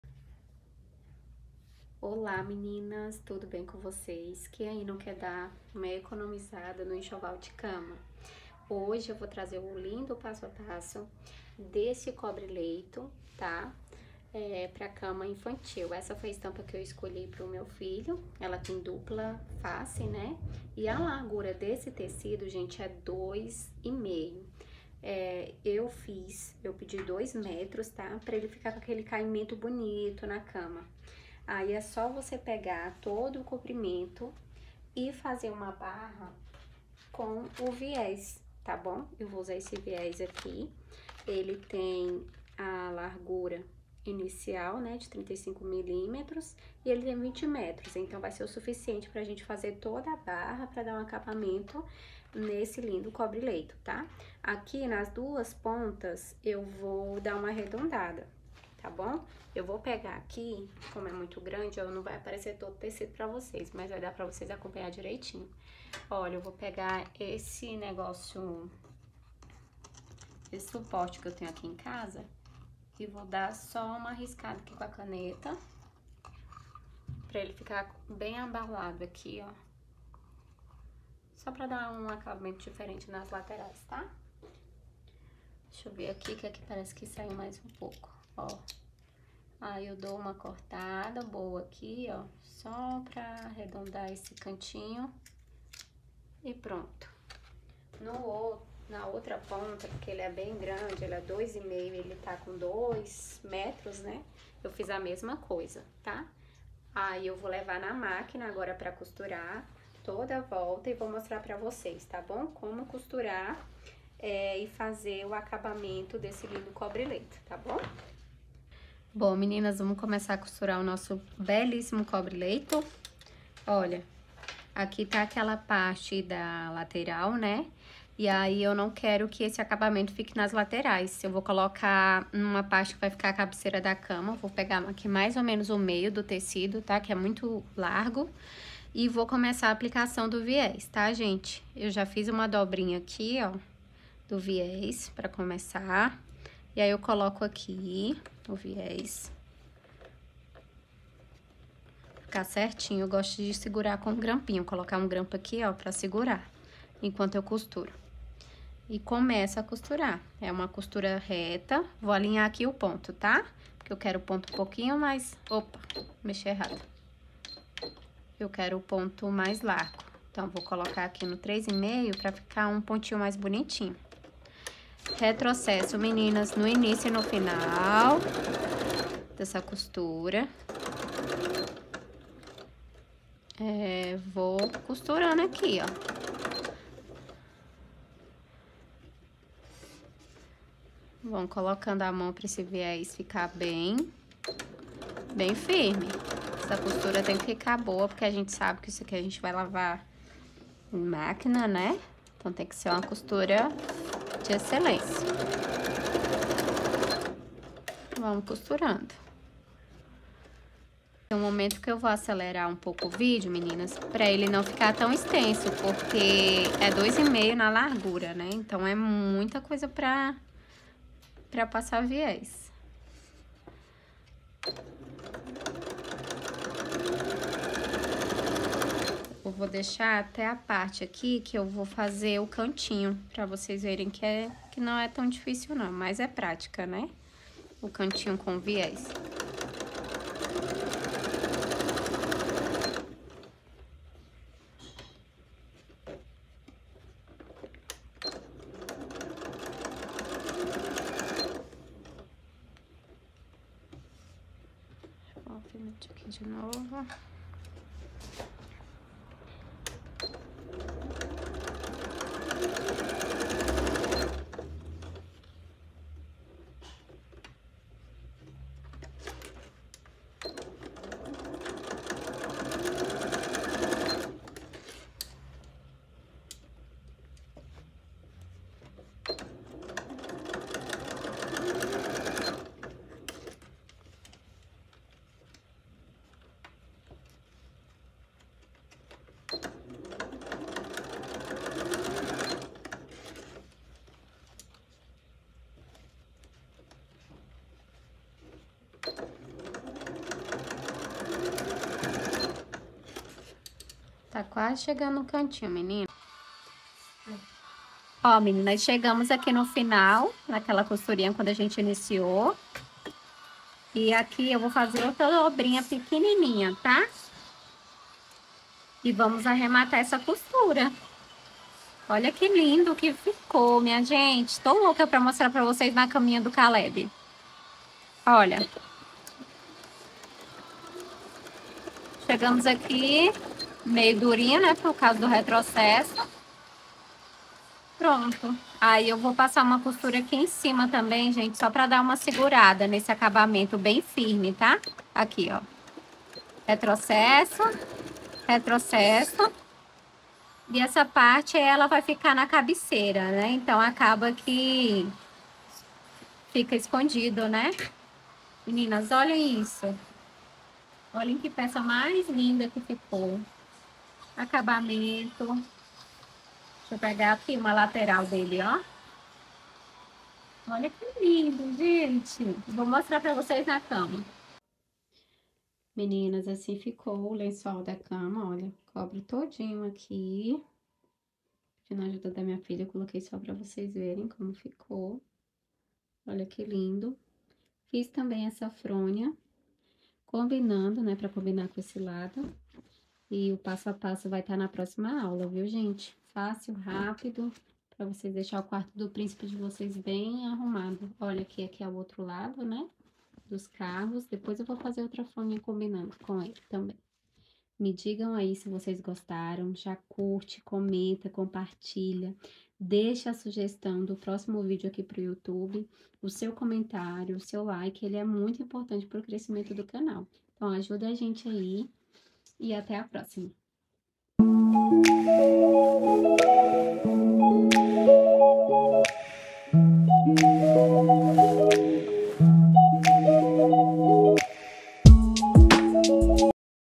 Como fazer um cobre-leito infantil gastando pouco- faça você mesmo
Oi meninas compartilhar uma dica de mãe para mãe, o fronde aproxima e nada melhor que uma cama quentinha e acolhedora para os nossos filhos não é mesmo? investi 70.00 reais e fiz esse cobre-leito de dois metros, com caimento maravilhoso, o tecido que usei está no vídeo espero que gostem.
Beijos de luz da Kelu🌷✨💕